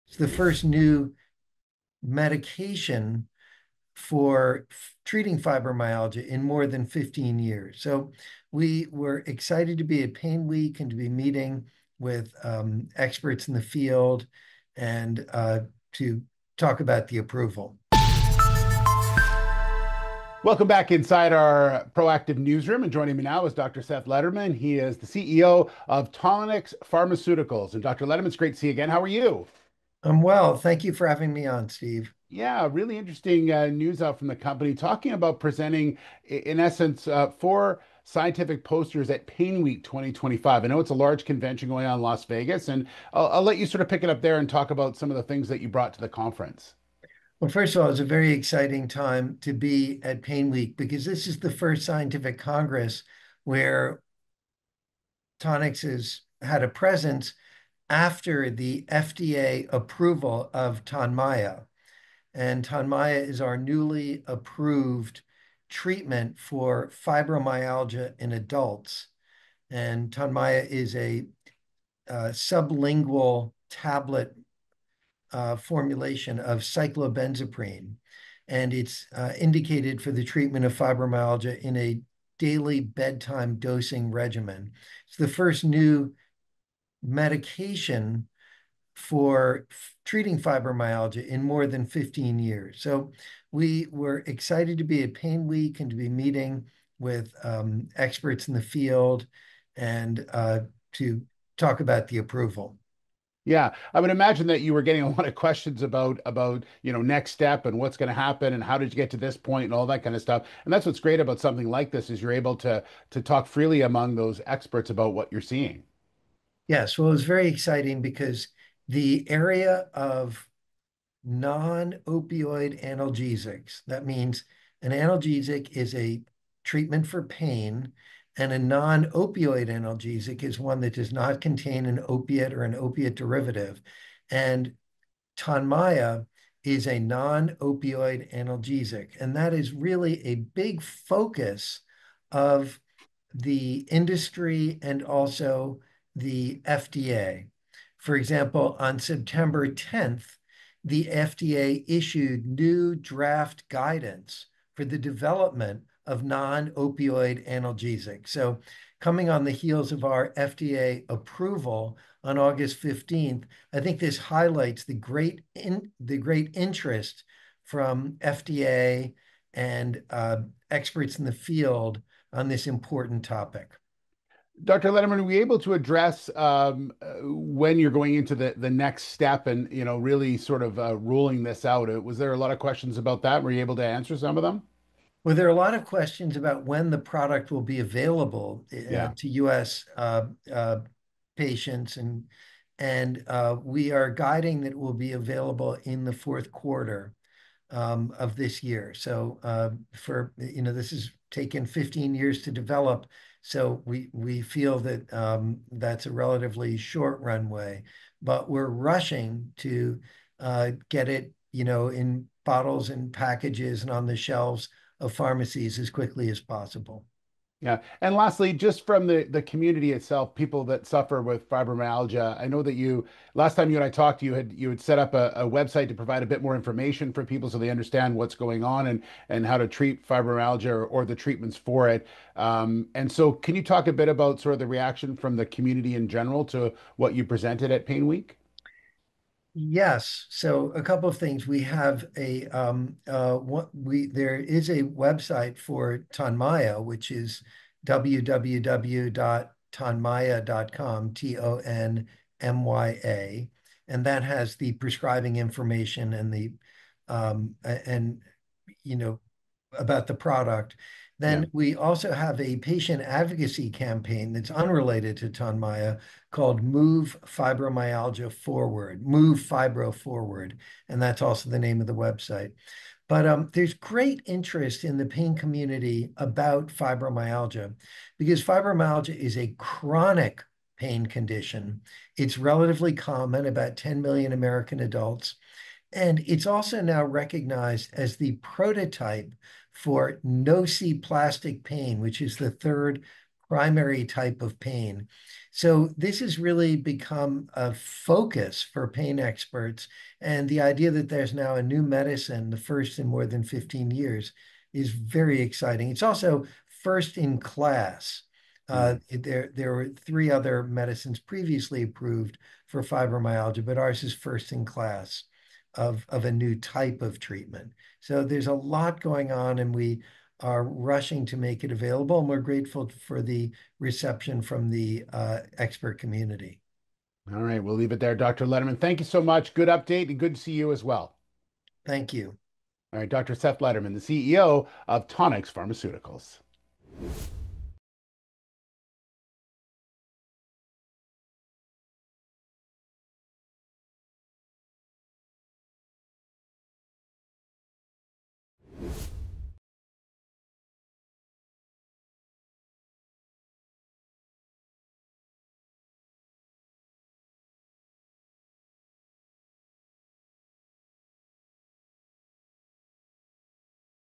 Tonix showcases Phase 3 data for FDA-approved Tonmya at PAINWEEK 2025, eyes launch next quarter
Tonix Pharmaceuticals Holdings CEO Dr Seth Lederman joined Steve Darling from Proactive to share that the company presented four scientific posters at the PAINWEEK 2025 conference in Las Vegas, Nevada, showcasing robust data from two pivotal Phase 3 trials of Tonmya® for the treatment of fibromyalgia.

The first study, RELIEF, was a 14-week randomized, double-blind, placebo-controlled trial, while the second, RESILIENT, served as a confirmatory efficacy and safety trial. Results across both studies demonstrated that Tonmya significantly reduced fibromyalgia-related pain while maintaining a favorable tolerability profile.

Unlike conventional pain therapies, Tonmya is designed to pharmacologically target nonrestorative sleep by antagonizing receptors that regulate sleep architecture—addressing a central mechanism thought to perpetuate fibromyalgia symptoms.

Dr. Lederman noted that beyond reducing pain, Tonmya’s mechanism of action may help improve a wide spectrum of fibromyalgia symptoms, potentially supporting earlier diagnosis, timely intervention, and improved long-term outcomes. Importantly, the therapy represents a non-opioid treatment option at a time when safer alternatives are in high demand.

With FDA approval secured, Tonix is now preparing to launch Tonmya commercially in the coming quarter, marking a transformative milestone for both the company and the fibromyalgia community.